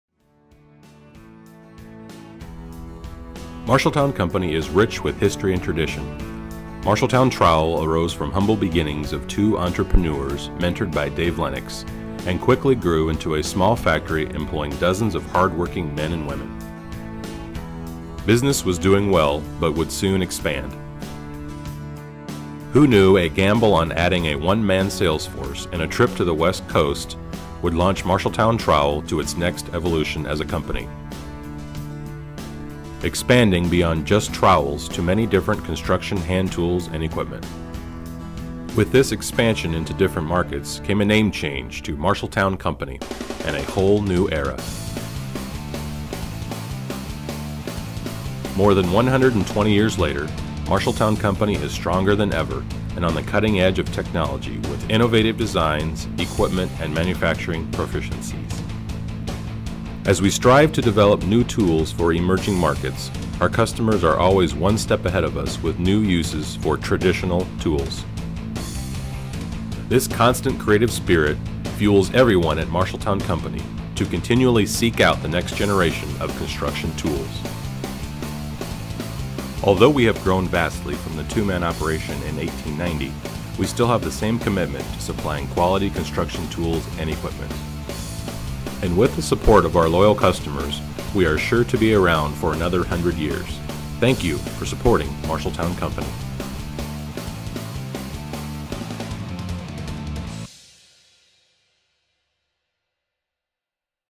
History of Marshalltown
A brief history of the Marshalltown Company - from our humble beginning in 1890 to the present day company. Marshalltown Company is more than a manufacturer of premium quality trowels. We are committed to leading the construction tool industry and providing the best products for the contractor to complete every job with the best results possible.